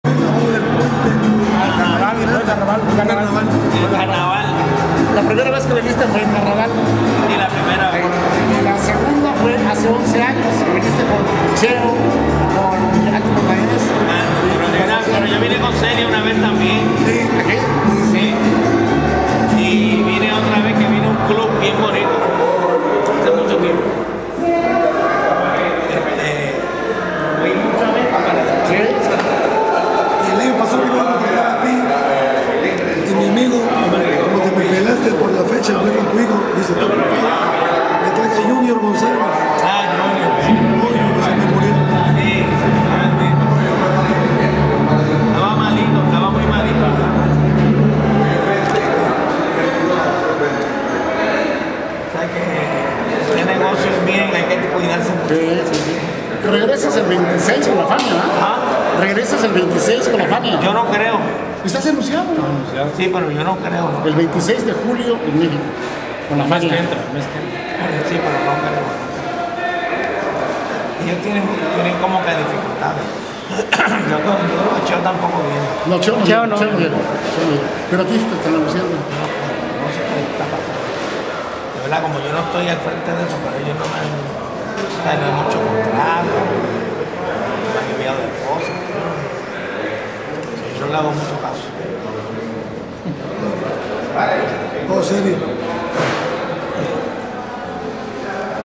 ISMAEL MIRANDA DECLARA QUE NO PARTICIPARA CON LA FANIA ALL STAR EN MEXICO
DURANTE SU VISITA AL PUERTO DE VERACRUZ ISMAEL MIRANDA AL LADO DE MR. SALSA DOMINGO LUCAS Y EL SEÑOR LORENZO PIEDRA MANIFESTO QUE NO VENDRA AL BAILE PROGRAMADO CON LAS ESTRELLAS DE FANIA EL 26 DE JULIO EN MEXICO.